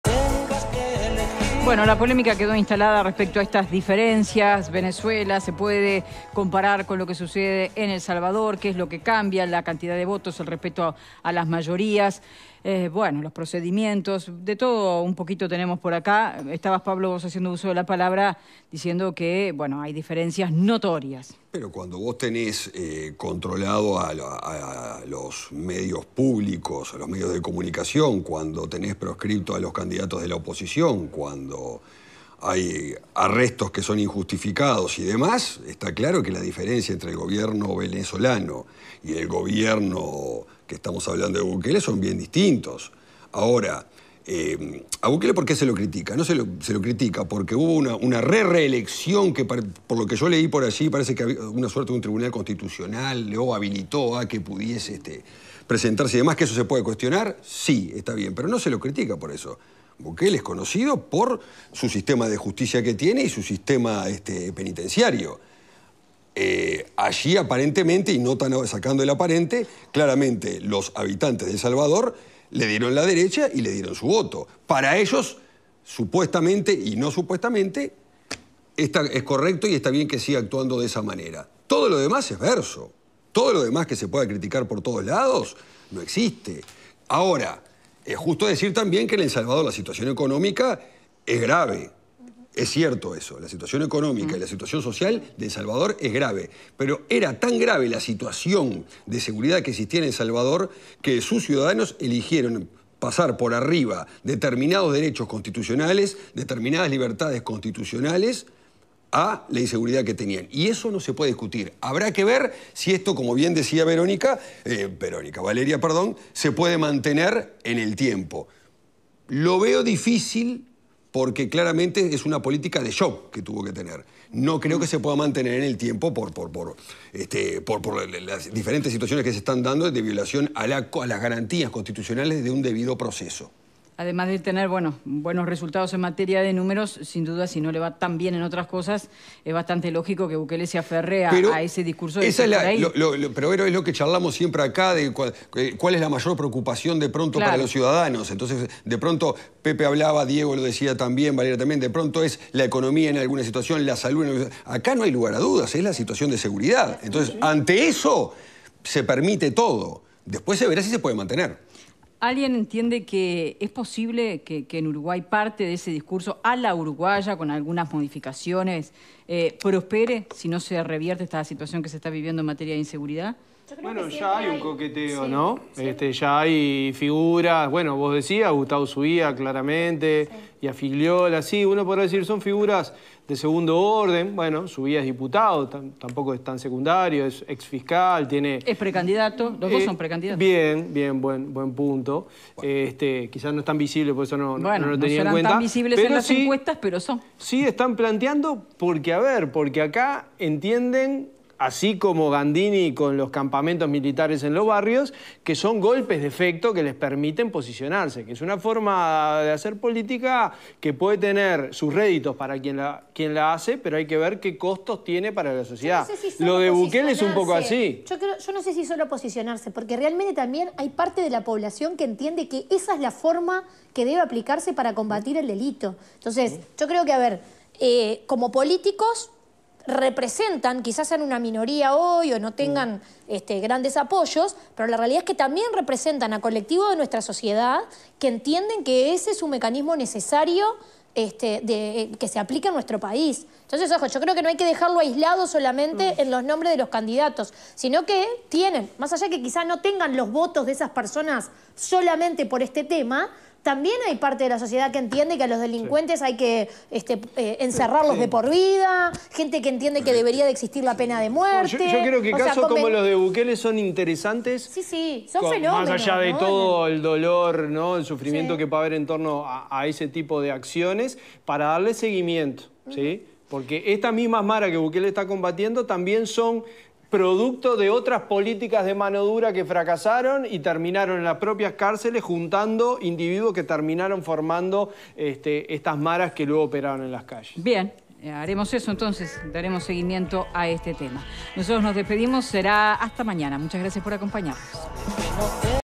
Nayib Bukele fue reelecto presidente de El Salvador
Debate sobre la estrategia de seguridad que impulsa.